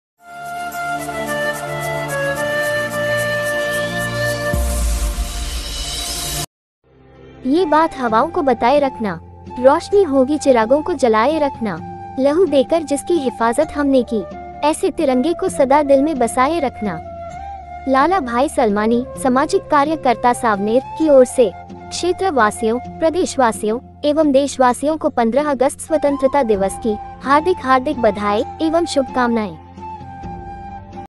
लाला भाई सलमानी समाजिक कार्यकर्ता सावनेर की ओर से स्वतंत्रता दिवस की शुभकामनाएं
लाला भाई सलमानी, समाजिक कार्यकर्ता सावनेर, की ओर से,

क्षेत्र वासियों, प्रदेश वासियों, एवं देश वासियों को 15 अगस्त स्वतंत्रता दिवस की, हार्दिक हार्दिक बधाई, एवं शुभकामनाएं